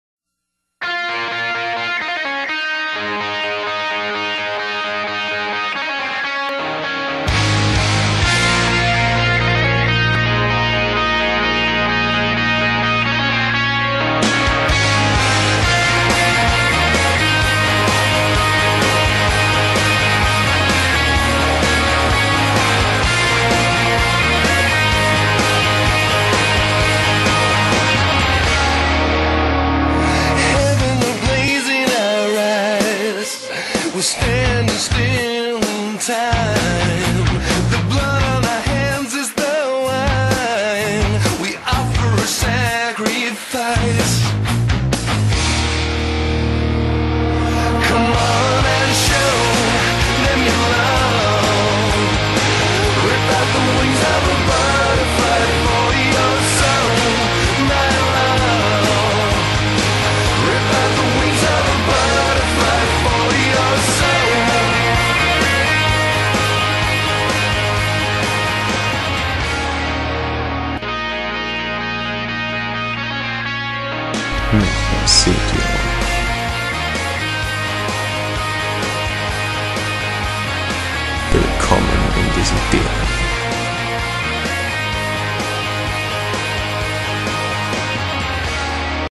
Desiderium [Best WoW Guild Ever]
Gilde:  Desiderium (lat. Sehnsucht)
Server:  Onyxia EU [Deutsch / German] - Jetzt auch auf Aman'thul! [Stand: September 2016]

[Wir sind uns darüber im klaren, dass HIM nur von Mädchen im Alter zwischen 10 und 15 Jahren sowie von homosexuellen Jungen gehört wird. Jedoch benötigten wir ein Lied, welches "Sehnsucht" ausdrückt, bzw. ein damit verbundenes Gefühl vermitteln lässt.]

Die original Tonspur ist deaktiviert, wodurch die kurzen Sprechphasen verloren gehen.